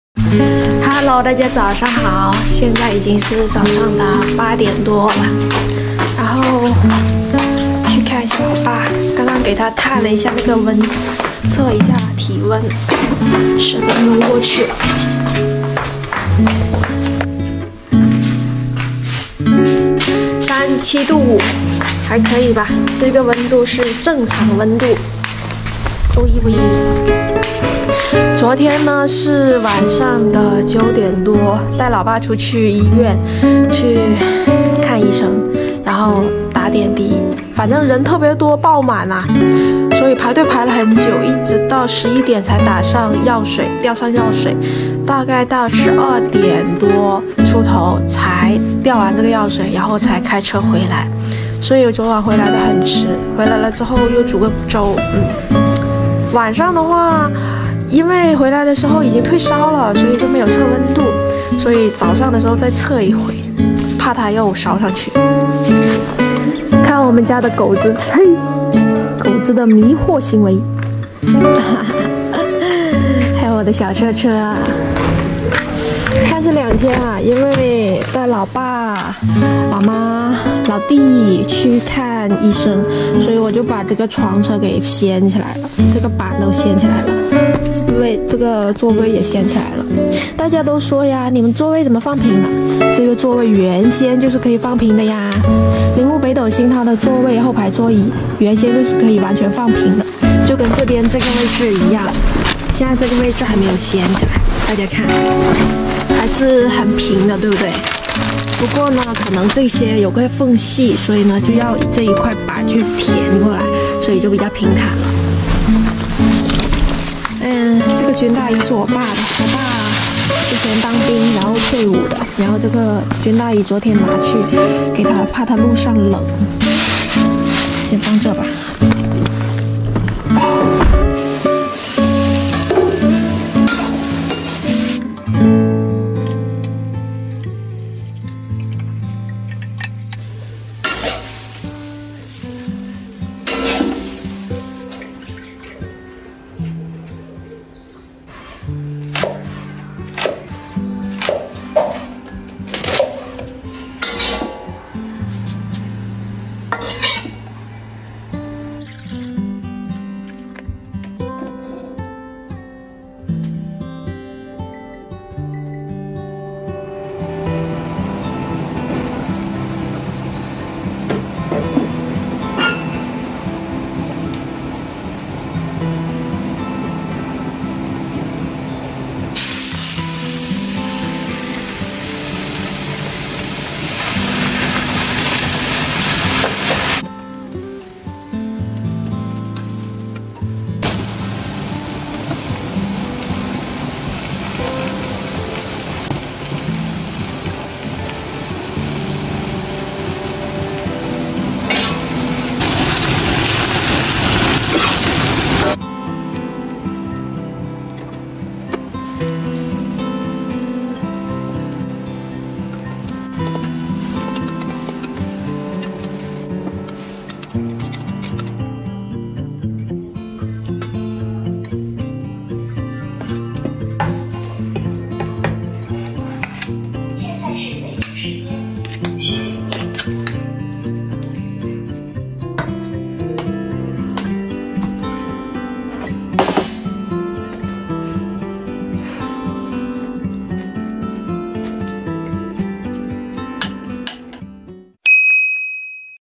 中国放开COVID新冠防控后，一家人都“阳了”，美女成了家里的顶梁柱！暂停旅行，没有抱怨，关键时候很靠谱！
一家人都阳了，我成了家里的顶梁柱！暂停旅行，关键时候很靠谱！